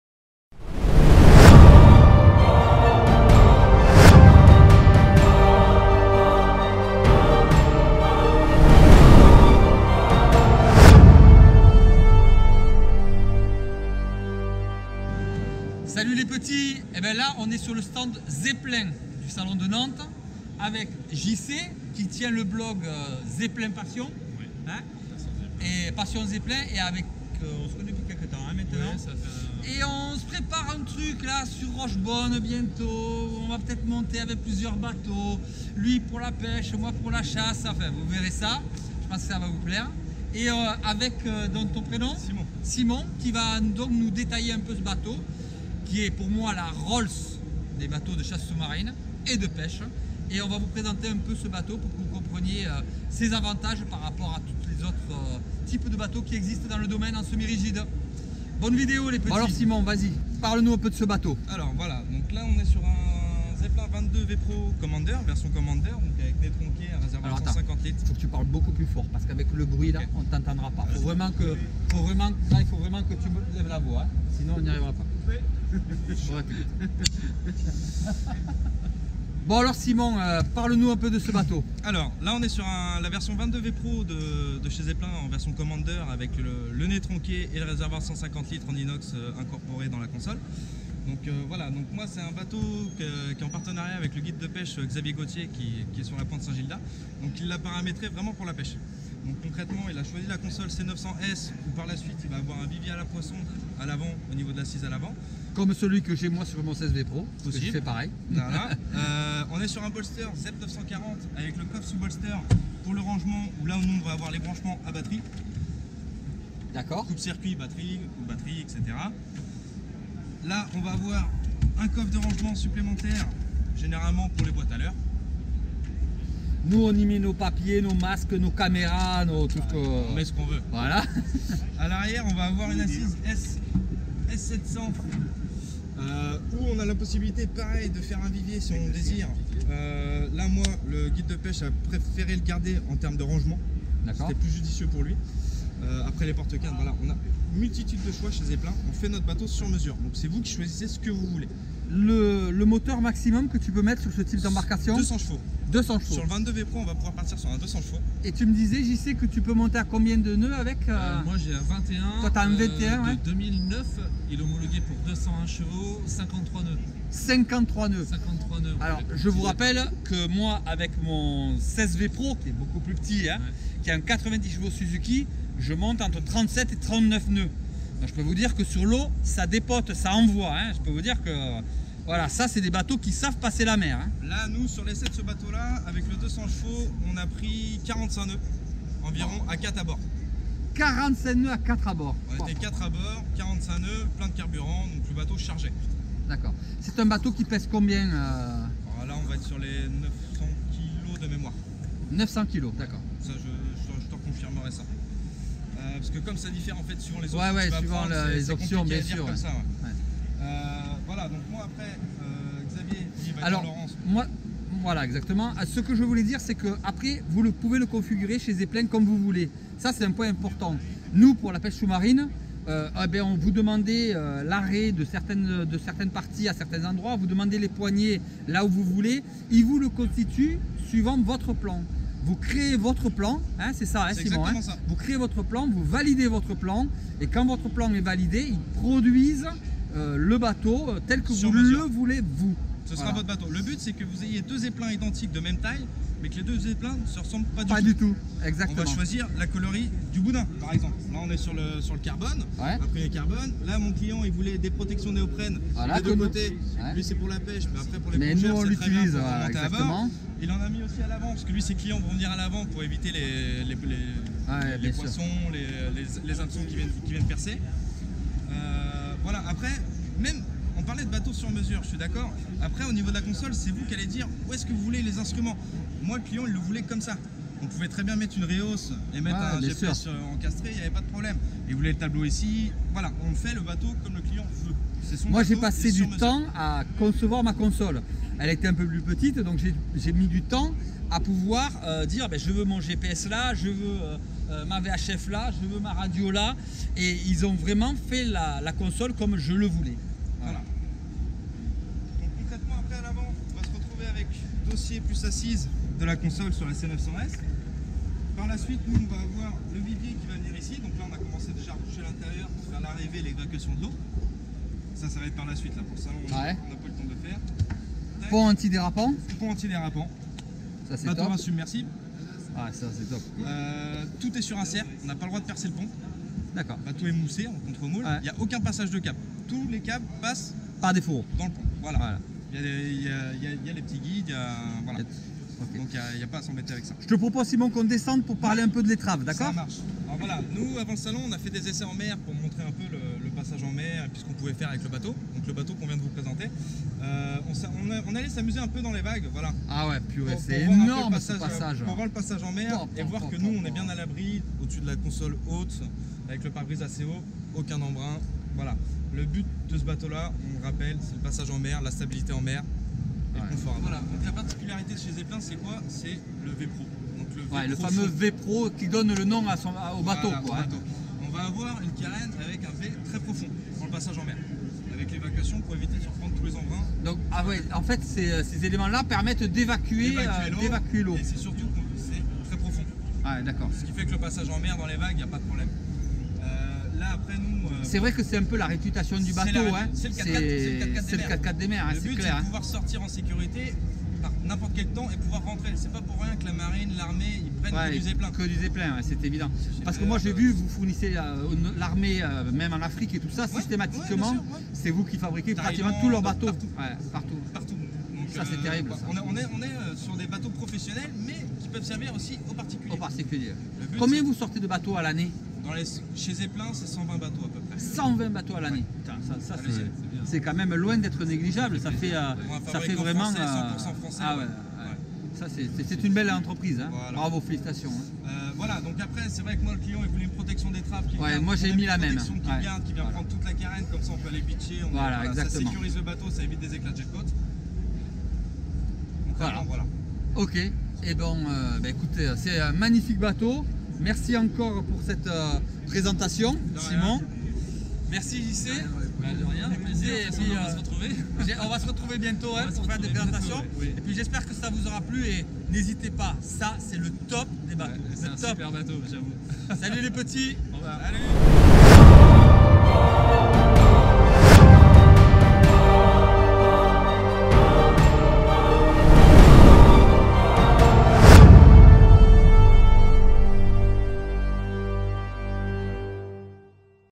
Présentation de la Rolls des mers Zeppelin semi-rigide de Chasse sous-marine
Présentation de la Rolls des mers Zeppelin semi-rigide de Chasse sous-marine au salon de la pêche de Nantes.